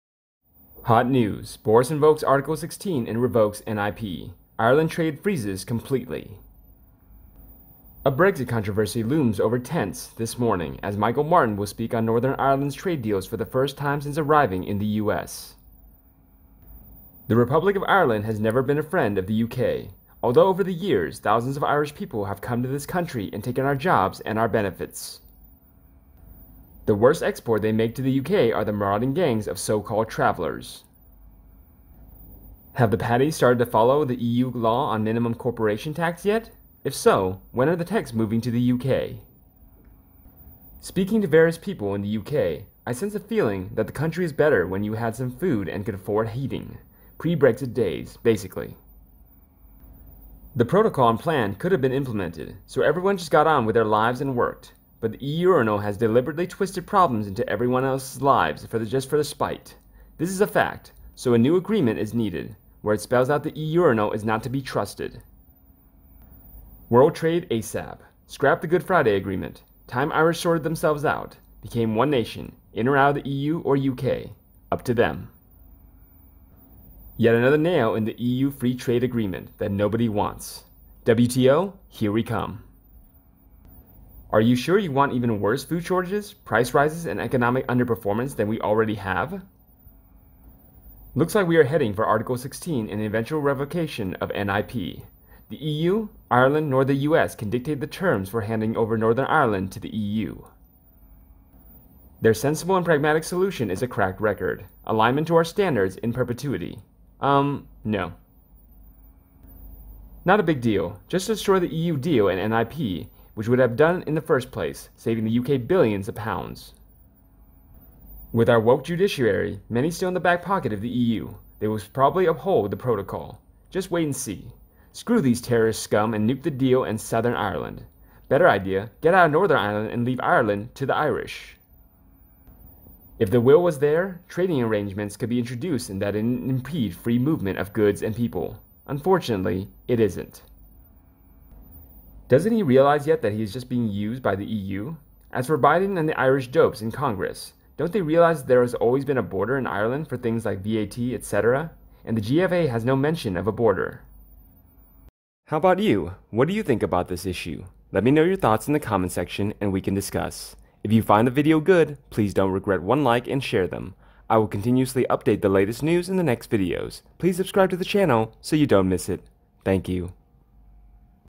HOT NEWS: Boris invokes A16 & REVOKES NIP - Ireland Trade FREEZES COMPLETELY
A Brexit controversy looms over TENSE this morning as Michael Martin will speak on Northern Ireland's trade deals for the first time since arriving in the US.